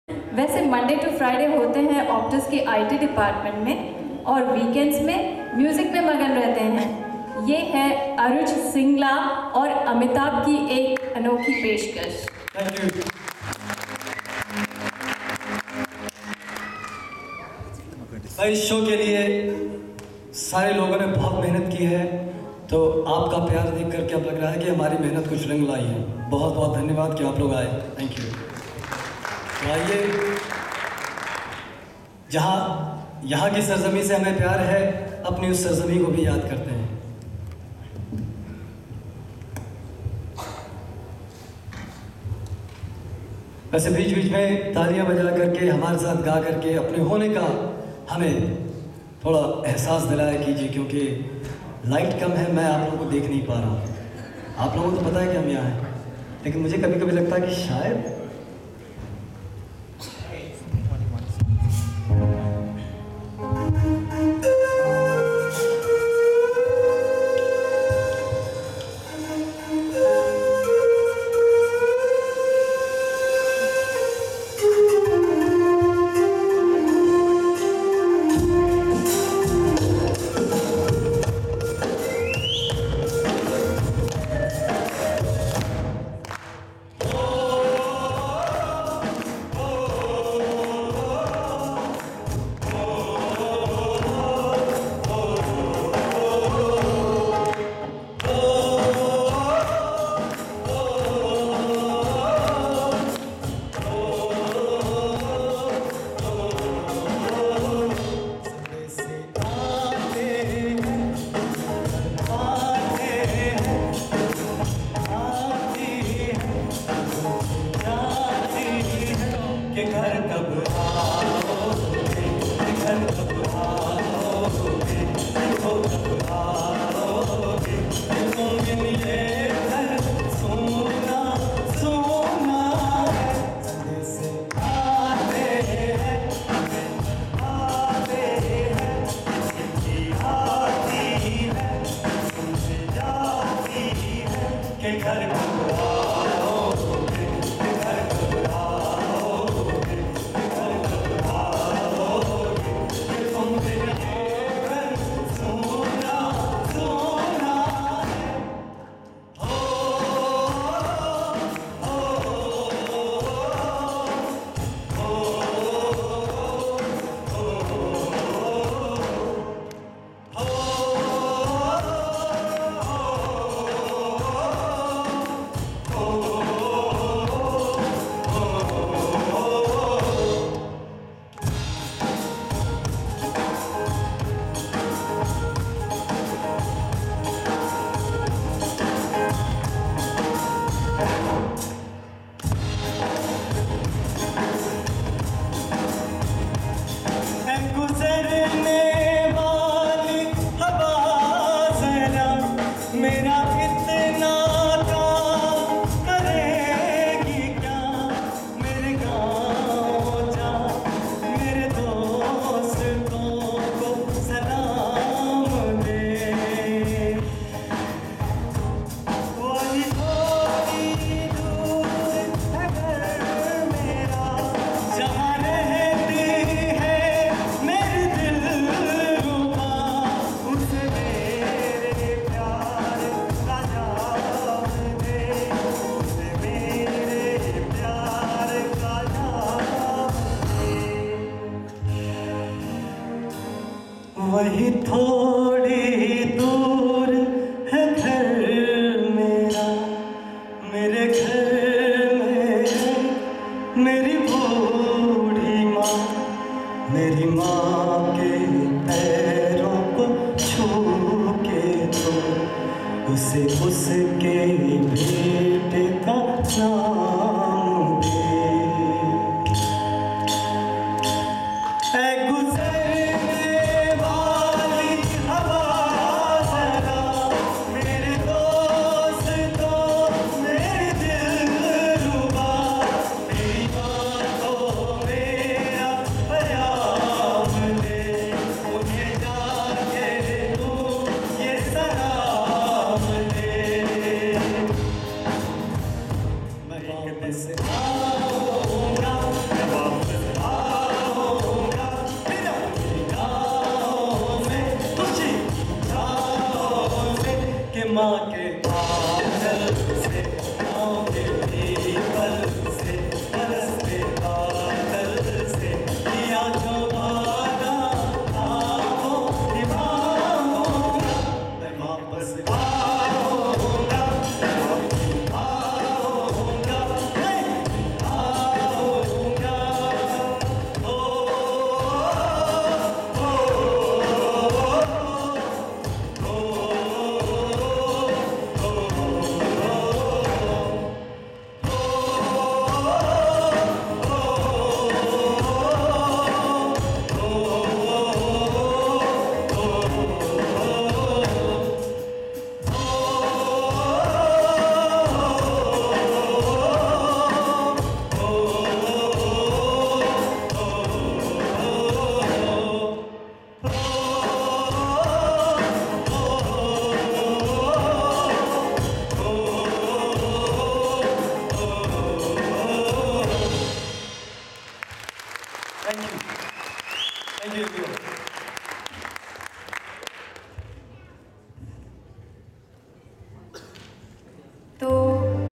Bollywood night 08
Singing with Amitabh Singh while performing with Om Music group Australia, at Bollywood night 08, held at Vic Uni St Albans Campus, melbourne